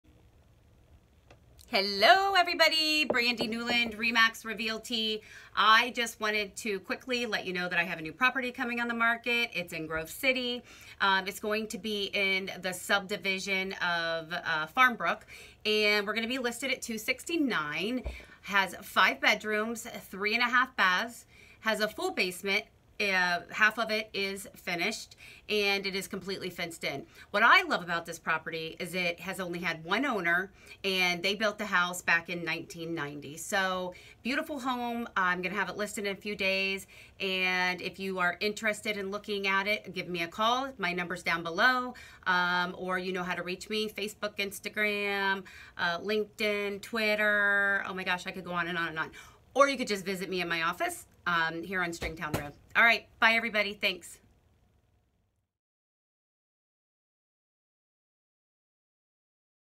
New Listing Alert in Grove City
New Listing Alert in Grove City.  The beautiful & mature neighborhood of Farmbrook.  This property has never been on the market because its been only 1 owner.  They have maintained this house so well & preserved it for the next homeowner.  It has 5 bedrooms & 3.5 bathrooms.  1 bedroom is on the 1st floor and it has a full bathroom however this is not the master suite.